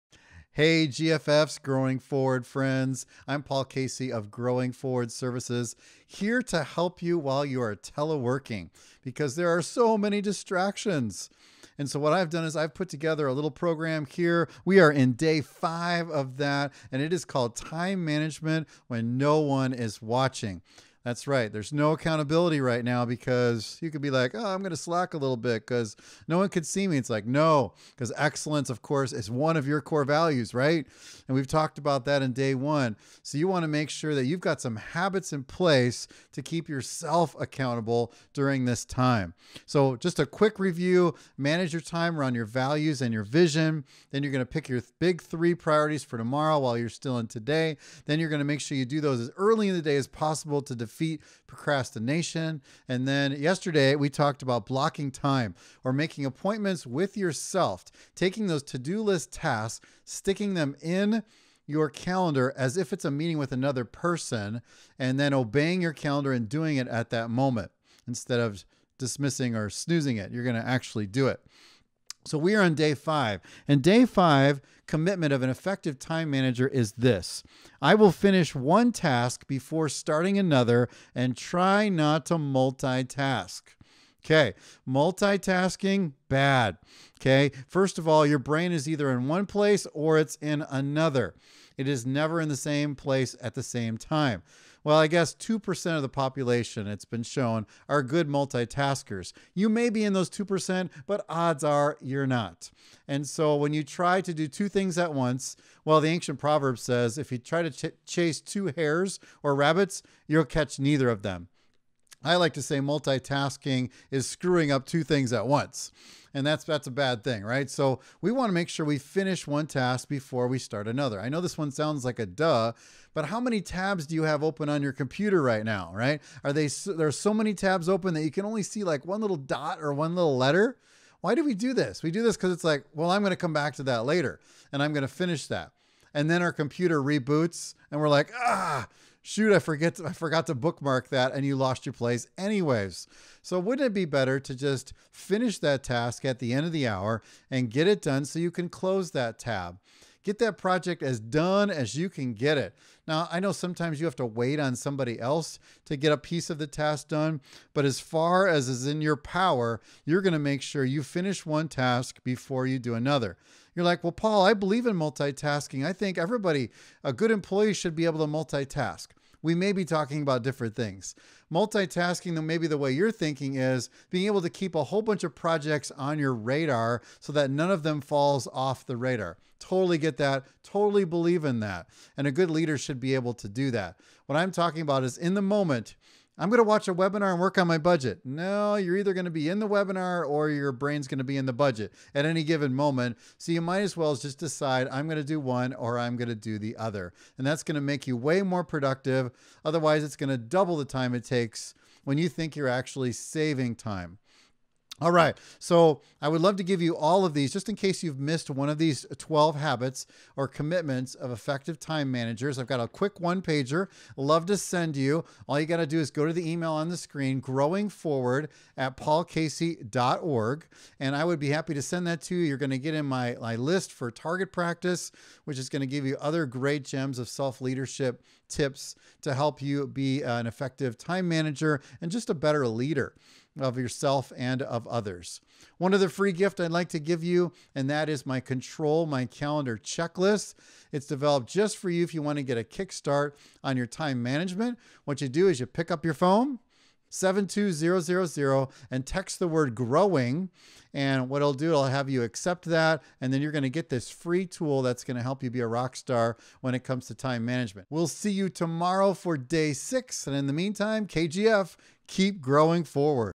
Time Management When No One Is Watching: Module Five - Finish That Task
On day 5 of this course, you now MUST finish a task before starting another. Don't fall for the multitasking fallacy. Do 1 single thing in this moment, and not something that splits your attention.

Text 72000 to Growing for the Control My Calendar Checklist